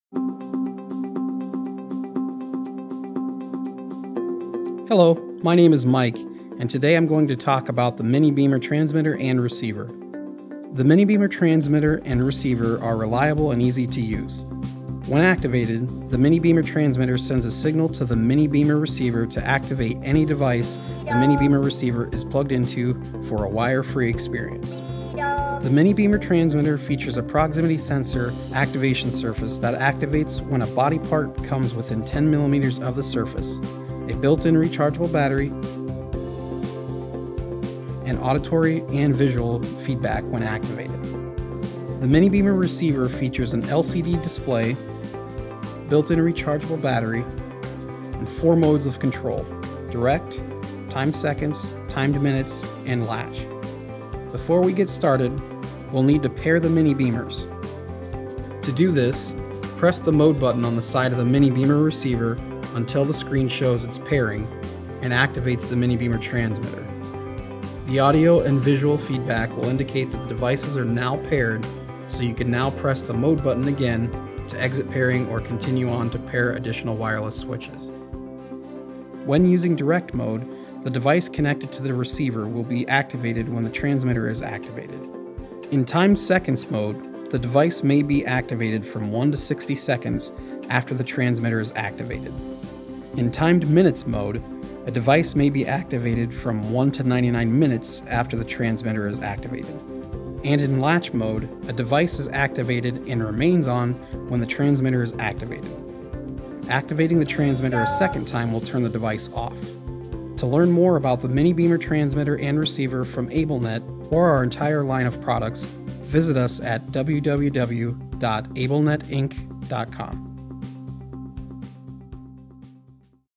AbleNet Technology Tidbits: Mini Beamer Transmitter and Receiver
A wireless switch kit featuring the Mini Beamer Transmitter and Mini Beamer Receiver gives you everything you need to start using a wireless switch.